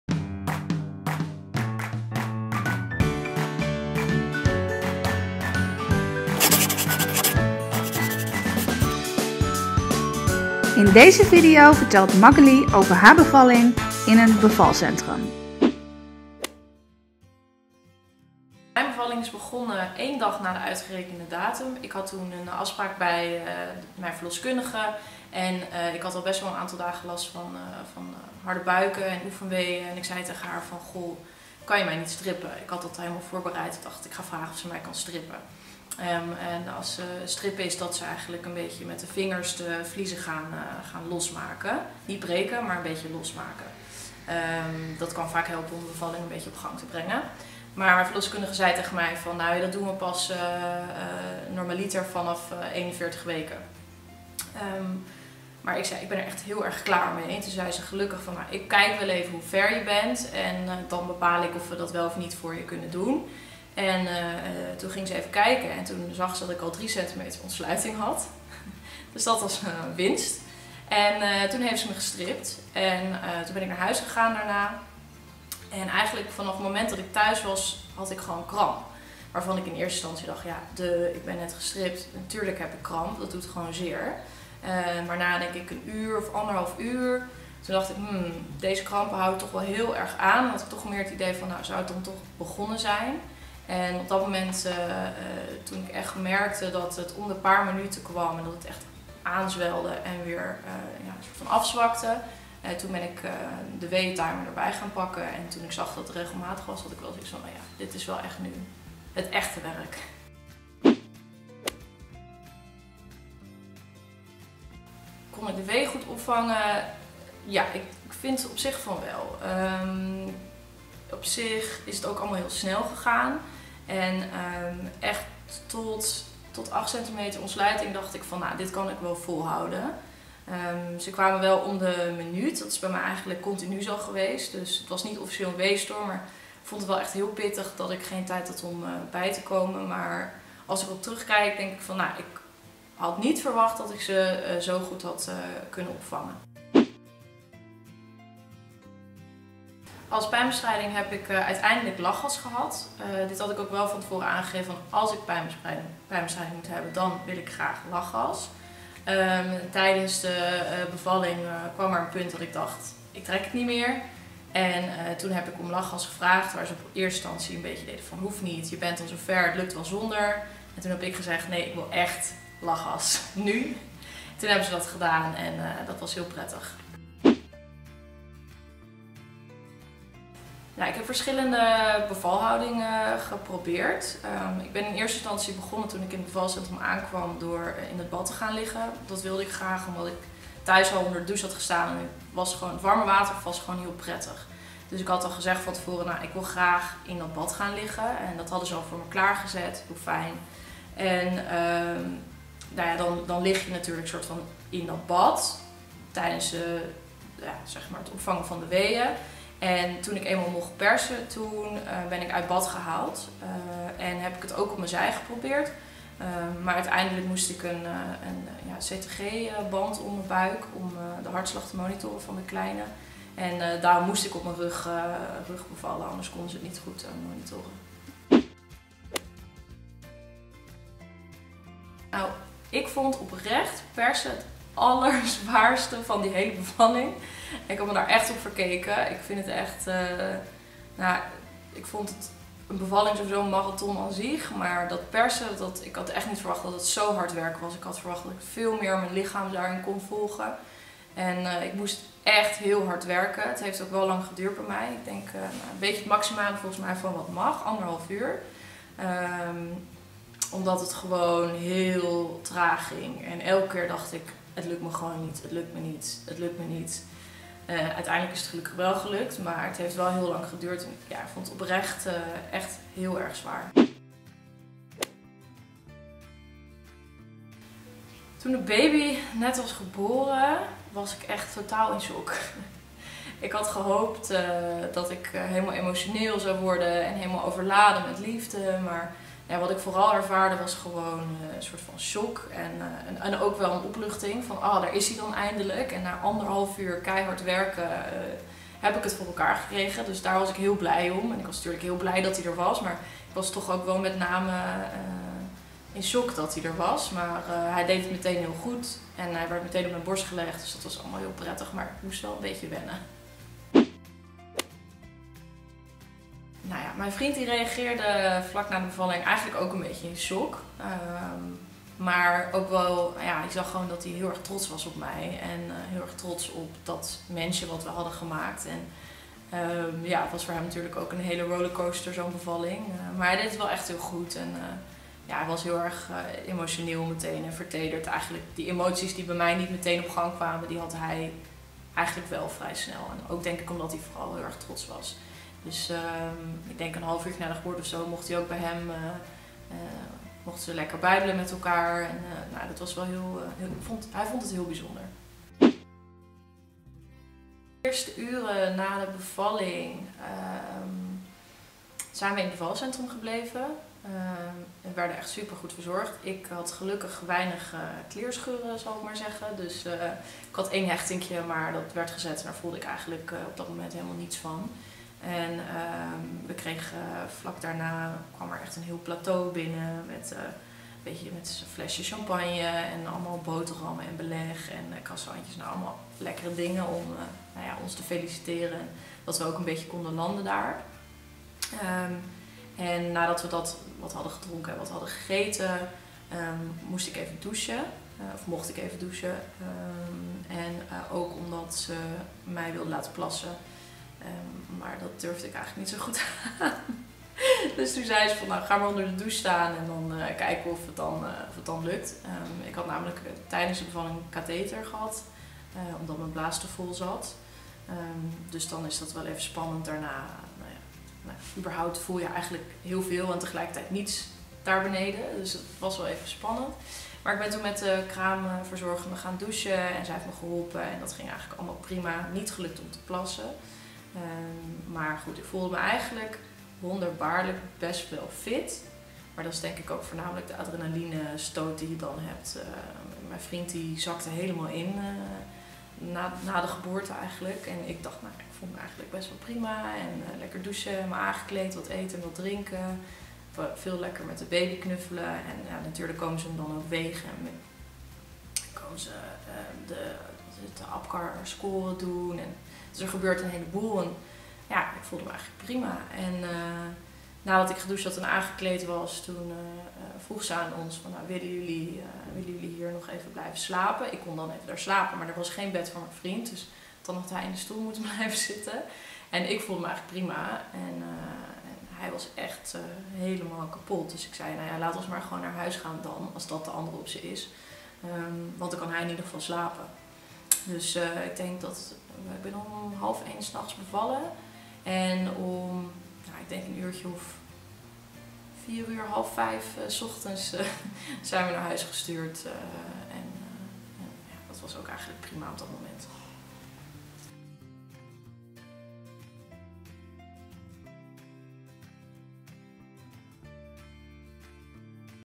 Mom to Mom | Magali over haar bevalling: "Ik was vooral in shock!"
Magali is bevallen in een bevalcentrum en heeft als pijnbestrijding lachgas gebruikt. Ze vertelt over de weeën, de persfase maar ook over haar reactie toen haar zoontje eenmaal was geboren.